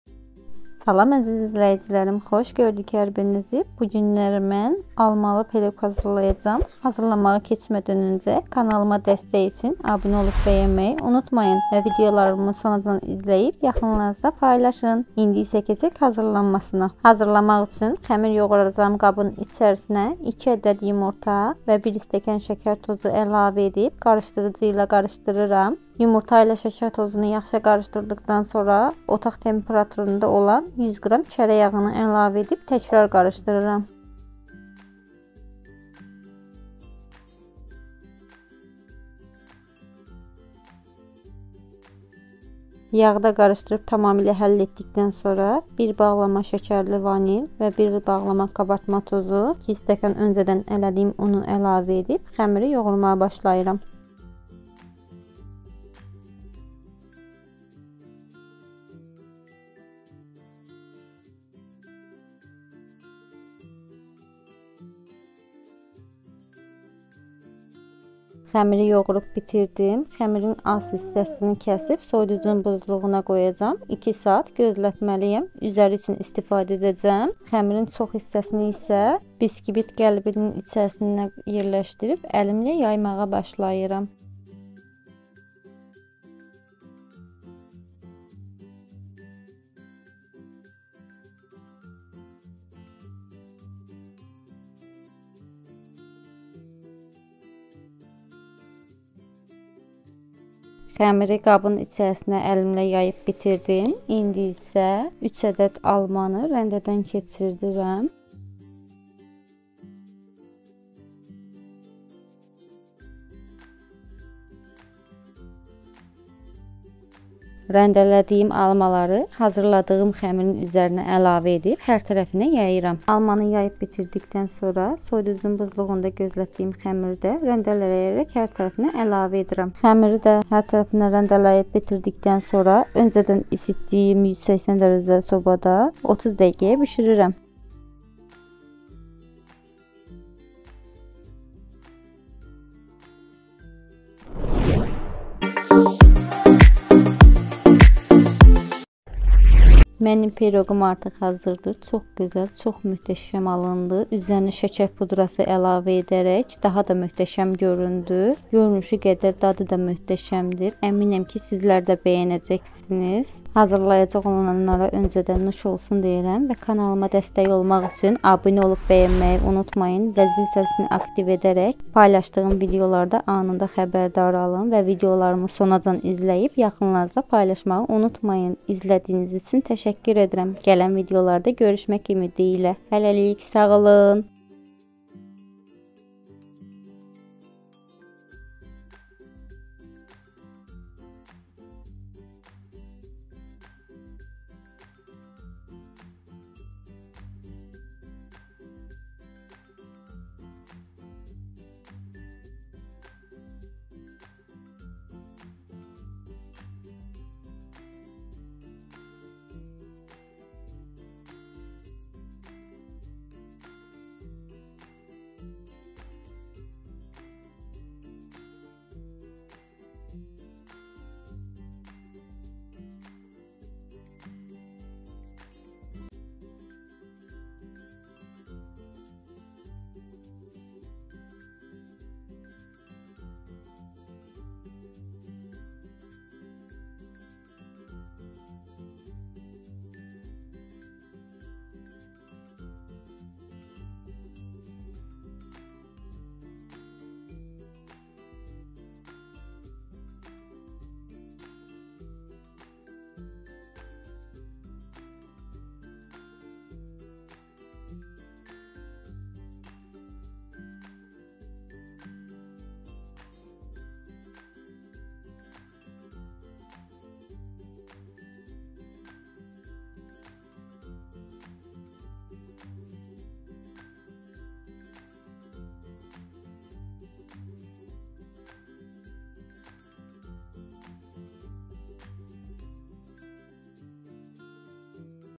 Alma piroqu çoxx dadlıdır mütləq hazırlayın peşman olmayacağsınız
Resepti
2. Ədəd yumurta
1. Stəkan şəkər tozu
100. Qram kərə yağı
1. Paket vanil
1. Paket kabartma tozu
2. Stəkan un

Üzəri üçün
3. Ədəd rəndələnmiş alma
Rəndələnmiş xəmir